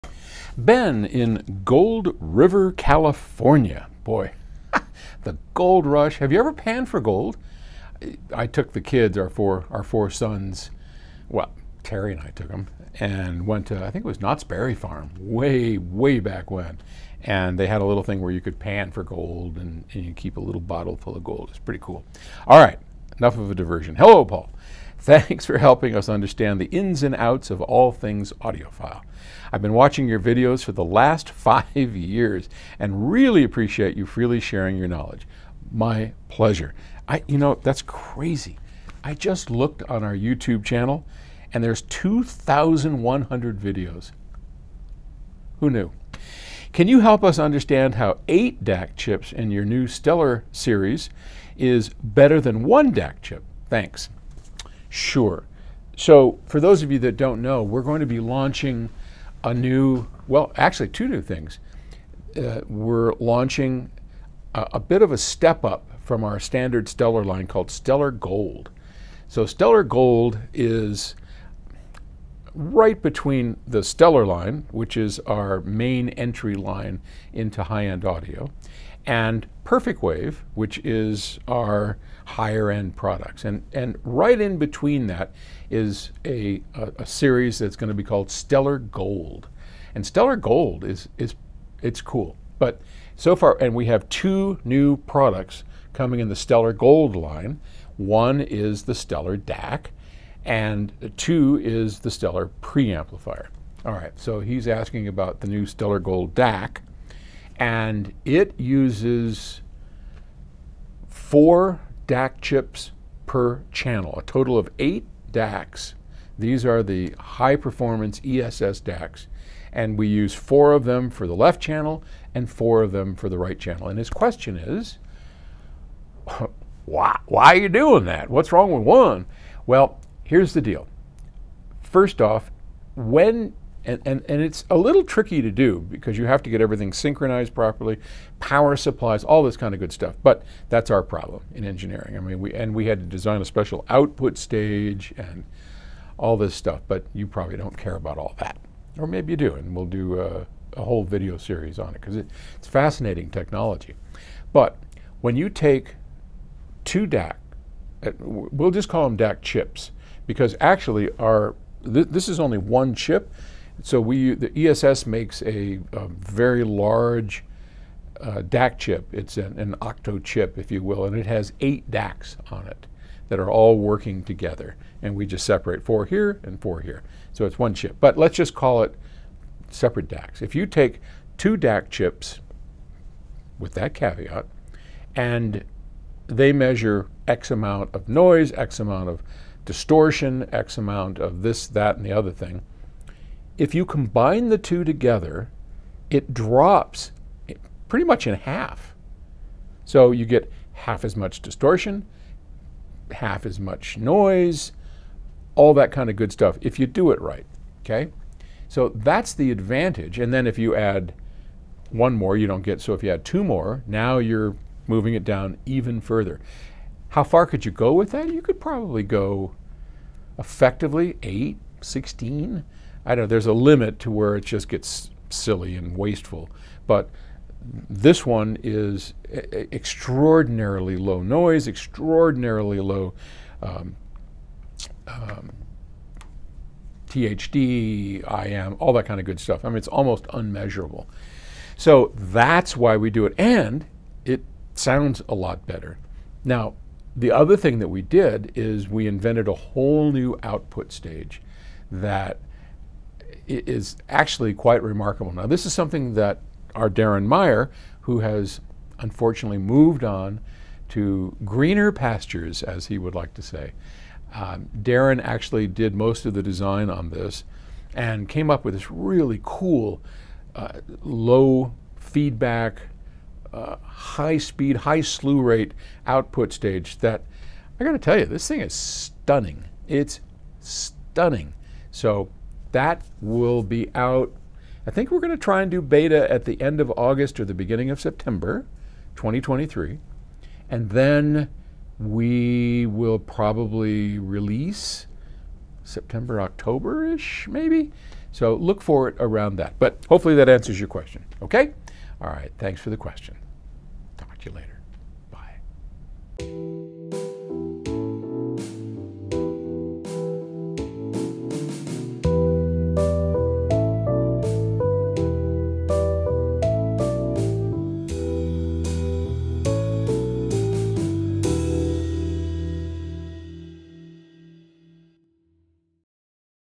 8 DAC chips in the new Stellar Gold
Why does PS Audio's latest new DAC, The Stellar Gold, have 8 DAC chips inside?